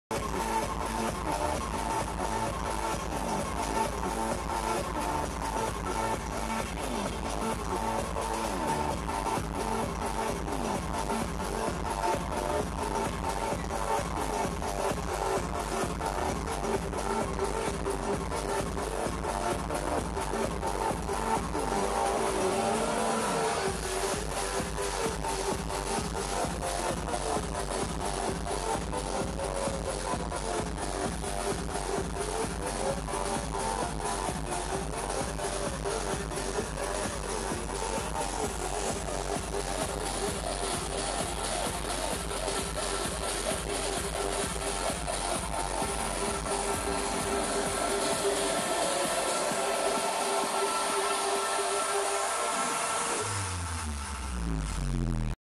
TIESTO w/ GoGo Gadgette Kel E @ LAN (Part 3/8)
The GoGo Gadgettes performing for LIGHTS ALL NIGHT!
Dallas Convention Center
12/30/2011